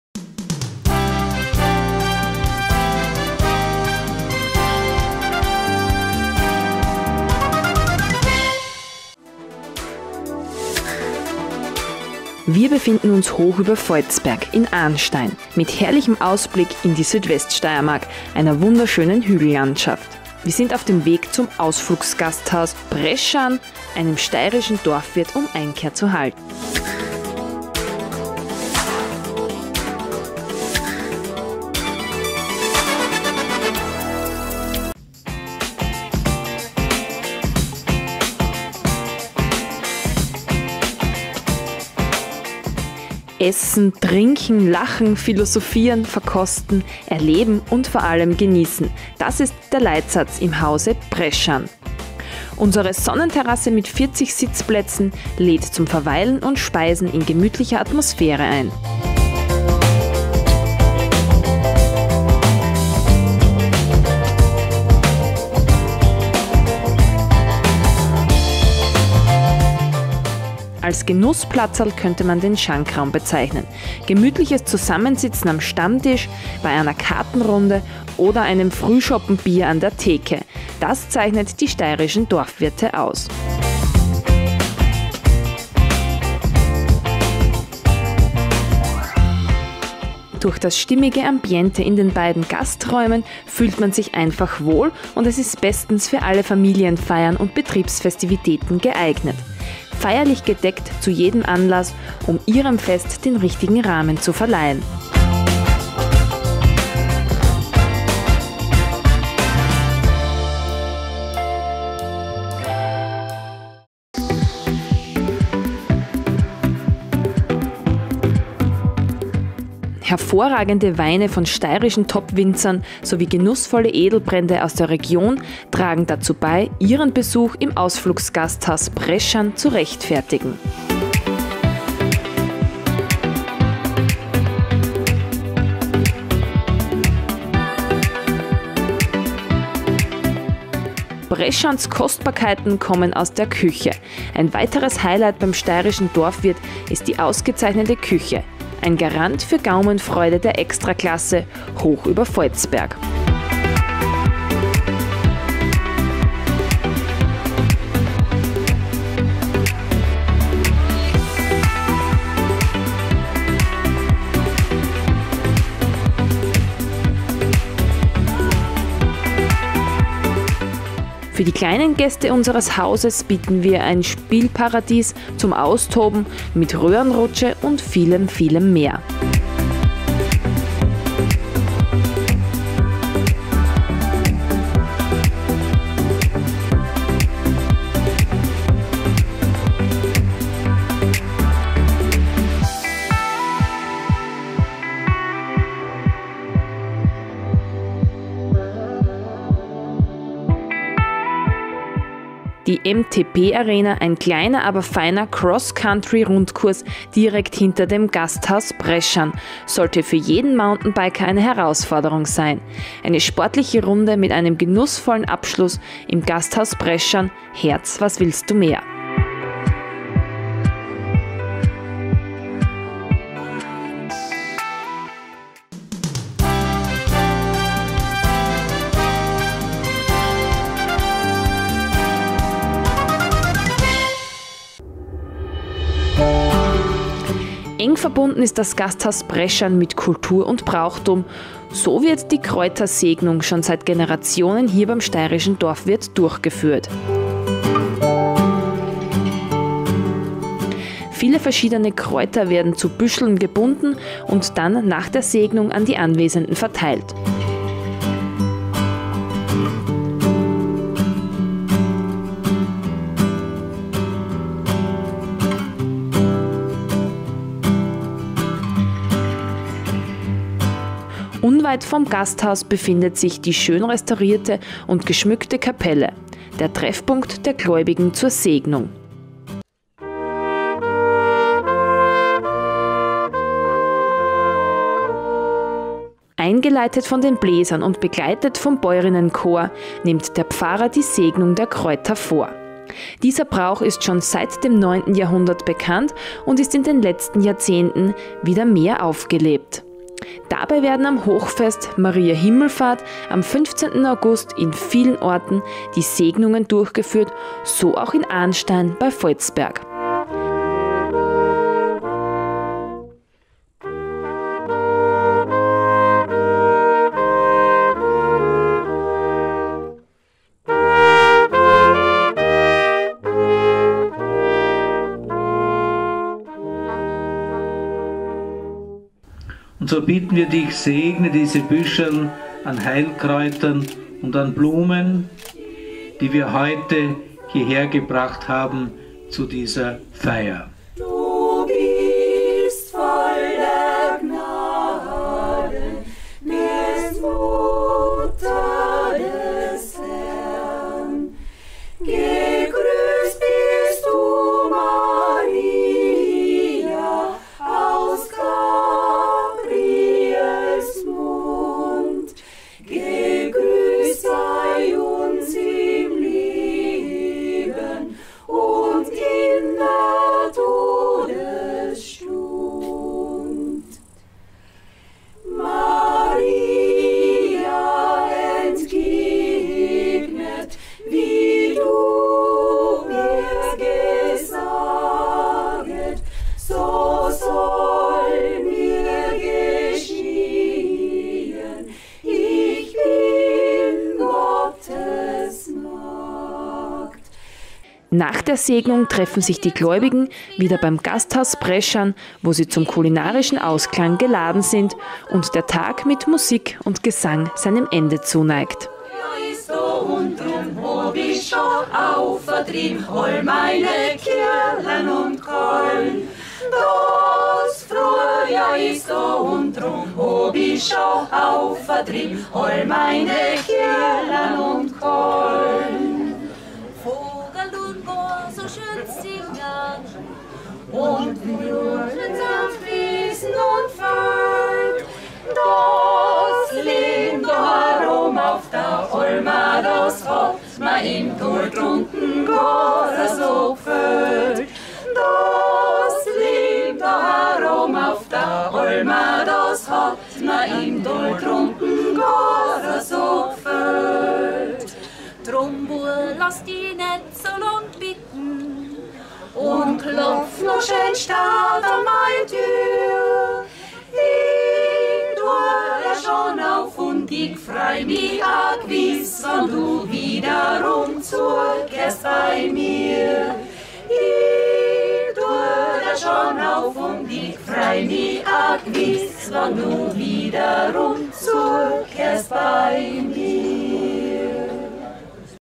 Hoch über Voitsberg: Gasthaus Preschan
Hier dreht sich alles um den Genuss: gutes Essen, ausgesuchte Weine und eine große Auswahl heimischer Edelbrände - beim Dorfwirt Preschan in Arnstein / Voitsberg. Kein Wunder, dass die traditionelle Kräutersegnung an Maria Himmelfahrt mit Musik und Gesang hier ihren krönenden Abschluss findet.
Besonders zu erwähnen sei noch die Sonnenterrasse des Gasthauses, das Spielparadies für Kinder und die der Mountainbike Cross Country Rundkurs direkt beim Gasthaus.